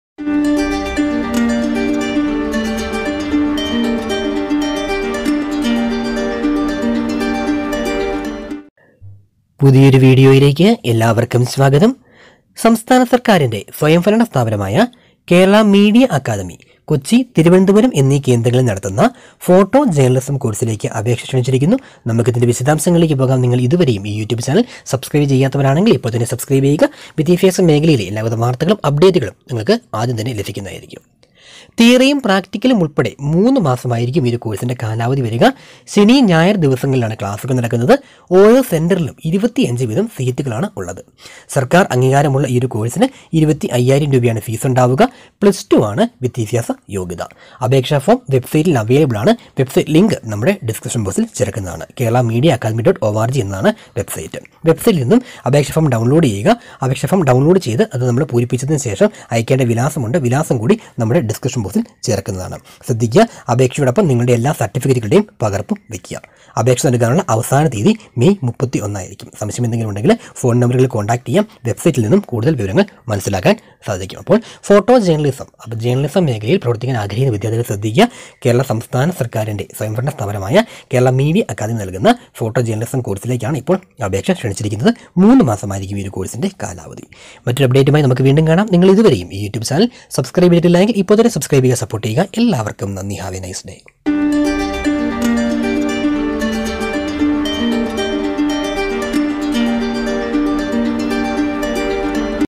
ഫോട്ടോ ജേര്‍ണലിസം കോഴ്‌സിന് അപേക്ഷ ക്ഷണിച്ചു | Kerala Media Academy | Apply Now | Edusoft Malayalam
ഫോട്ടോ ജേര്‍ണലിസം കോഴ്‌സിന് അപേക്ഷ ക്ഷണിച്ചു

സംസ്ഥാന സര്‍ക്കാരിന്റെ സ്വയംഭരണസ്ഥാപനമായ കേരള മീഡിയ അക്കാദമി കൊച്ചി, തിരുവനന്തപുരം സെന്ററുകളില്‍ നടത്തുന്ന ഫോട്ടോ ജേര്‍ണലിസം കോഴ്‌സിന് അപേക്ഷ ക്ഷണിച്ചു.

തിയറിയും പ്രാക്ടിക്കലും ഉള്‍പ്പെടെ മൂന്ന് മാസമാണ് കോഴ്‌സിന്റെ കാലാവധി. ശനി, ഞായര്‍ ദിവസങ്ങളിലാണ് ക്ലാസുകള്‍.

ഓരോ സെന്ററിലും 25 സീറ്റാണുള്ളത്. സര്‍ക്കാര്‍ അംഗീകാരമുള്ള കോഴ്‌സിന് 25000 രൂപയാണ് ഫീസ്.

പ്ലസ് ടു വിദ്യാഭ്യാസയോഗ്യതയുള്ളവര്‍ക്ക് അപേക്ഷിക്കാം. അപേക്ഷ അക്കാദമി വെബ്‌സൈറ്റില്‍ നിന്നു ഫോറം ഡൗണ്‍ലോഡ് ചെയ്ത് സമര്‍പ്പിക്കാം.

 അപേക്ഷ അയയ്‌ക്കേണ്ട വിലാസം (സെക്രട്ടറി, കേരള മീഡിയ അക്കാദമി, കാക്കനാട്, കൊച്ചി - 30 / കേരള മീഡിയ അക്കാദമി സബ്‌സെന്റര്‍, ശാസ്തമംഗലം, ഐ.സി.െഎ.സി.ഐ ബാങ്കിന് എതിര്‍വശം, തിരുവനന്തപുരം - 10).

അപേക്ഷയോടൊപ്പം സര്‍ട്ടിഫിക്കറ്റുകളുടെ പകര്‍പ്പും വയ്ക്കണം. അപേക്ഷ സ്വീകരിക്കുന്ന

അവസാന തീയതി 2022 മെയ് 31.

കൂടുതല്‍ വിവരങ്ങള്‍ക്ക് ഫോണ്‍: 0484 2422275, 2422068, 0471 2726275

kuhs university news